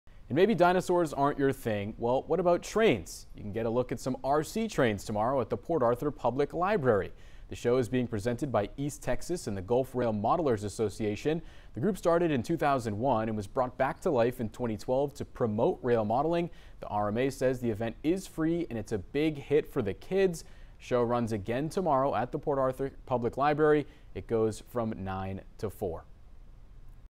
Locomotive lovers can see model train show at Port Arthur Public Library
The event is free.